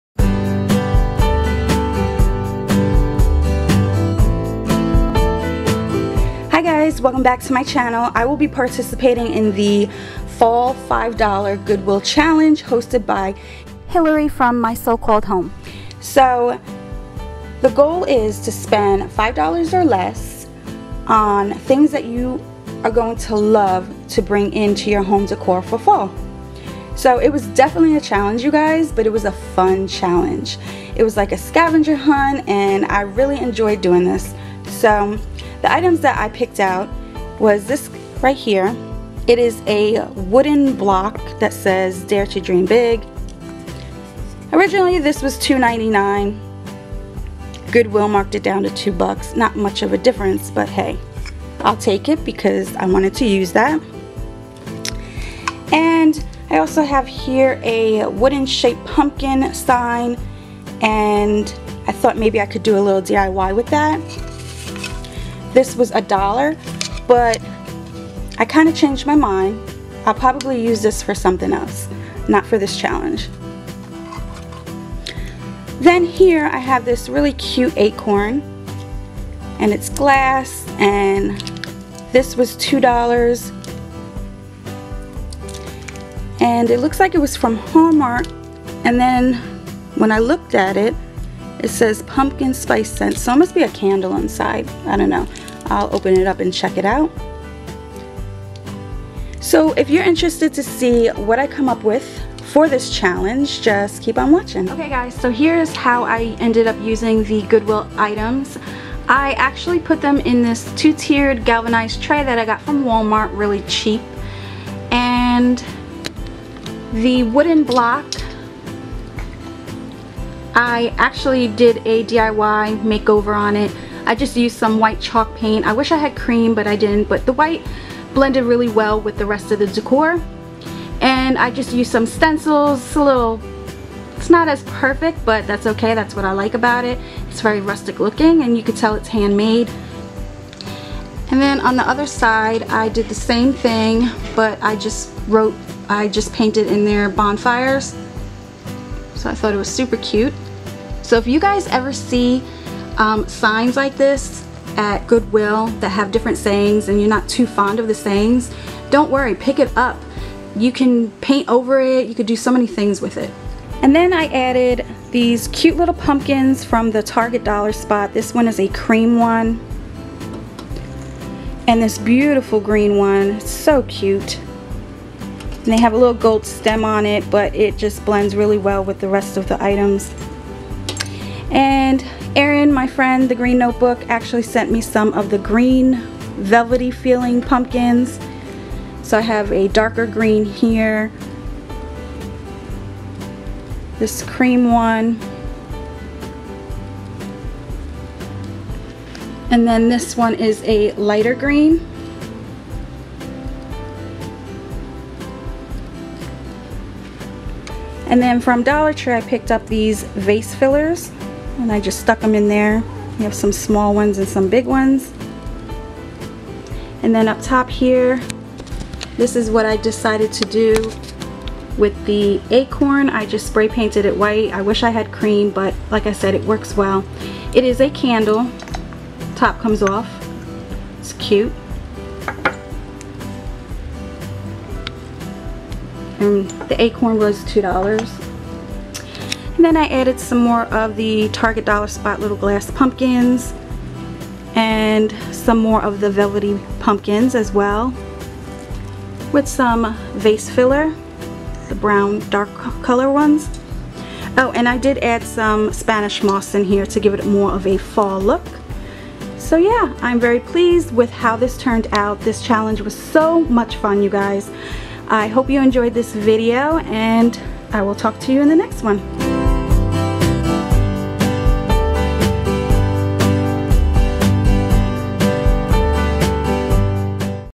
$5 GOODWILL CHALLENGE|FALL 2017
$5 Fall Goodwill Challenge hosted by Hillary from my so-called home